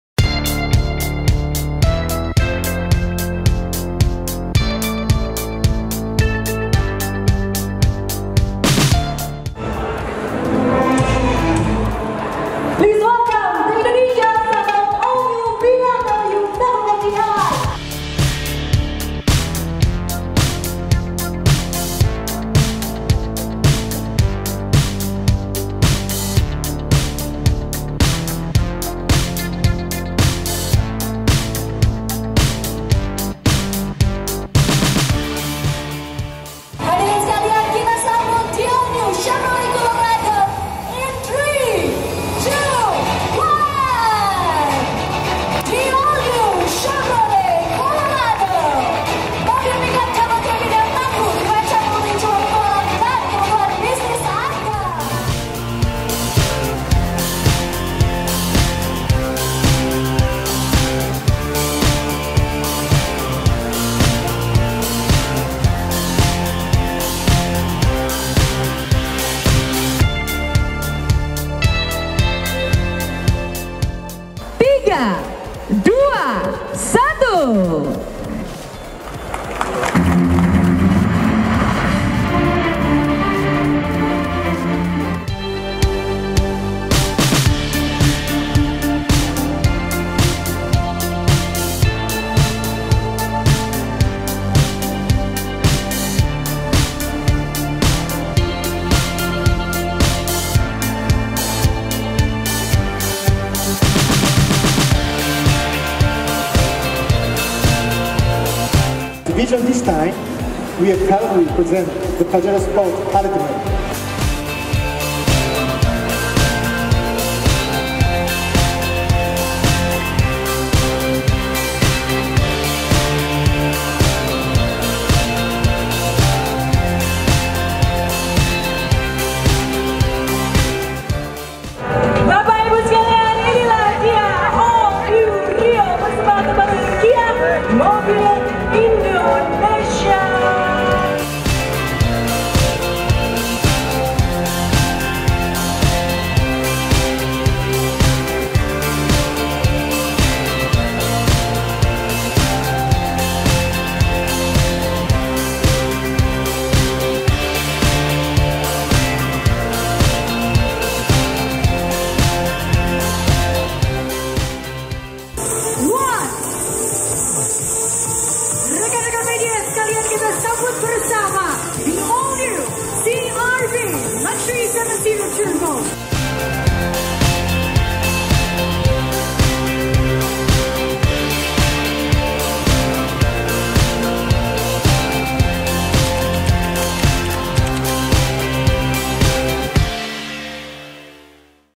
IIMS 2017 | Peluncuran 6 Mobil Baru | OTO.com
Indonesia International Motor Show 2017 Perhelatan akbar yang berlangsung di JIEXPO Kemayoran, Jakarta diselenggarakan hingga 8 Mei 2017, dengan menjanjikan kemeriahan yang lebih dari tahun sebelumnya.

IIMS merupakan tempat untuk melihat dan mendapatkan mobil baru, Tercatat 6 merek mobil baru meluncur di acara ini. Tidak heran, para pabrikan mobil membawa serta beragam promo menarik bagi mereka yang membeli mobil.

Dyandra Promosindo menargetkan IIMS menjadi tempat perputaran uang dengan transaksi tidak kurang dari Rp 3,1 triliun. Sebuah angka yang logis mengingat banyaknya promosi pembelian produk otomotif bagi konsumen.